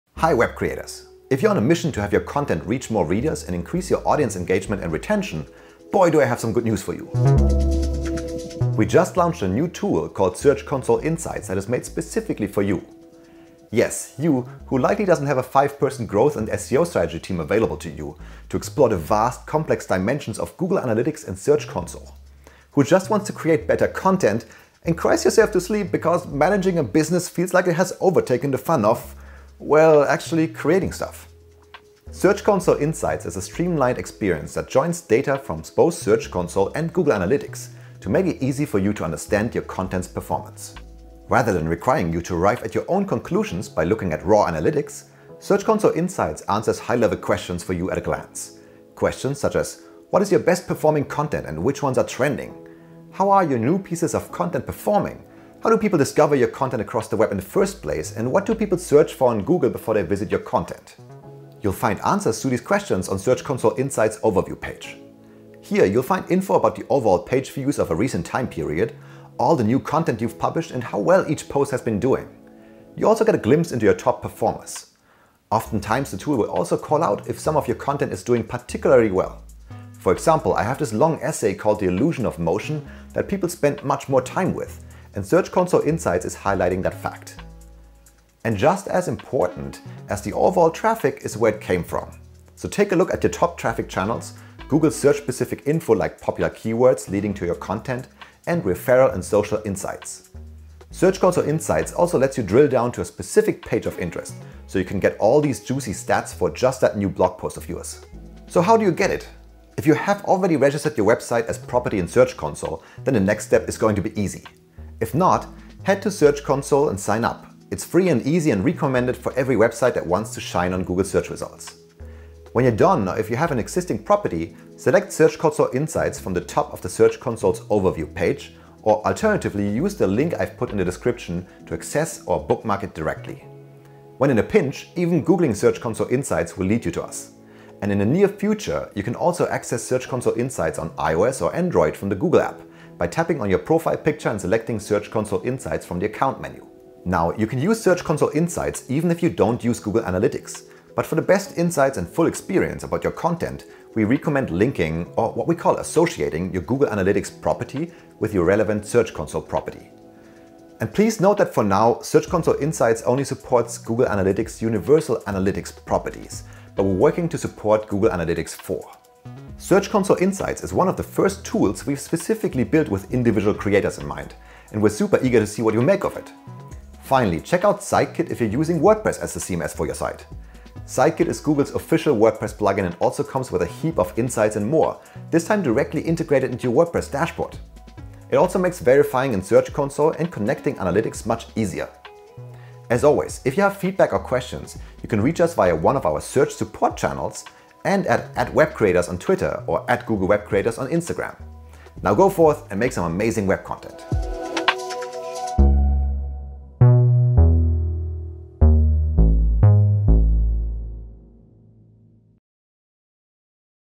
Understand Your Data With Search Console Insights
Google is launching Search Console Insights, a new experience tailored for content creators and publishers that can help them clearly understand how audiences discover their site’s content and what resonates with their audiences. This new experience is powered by data from both Google Search Console and Google Analytics.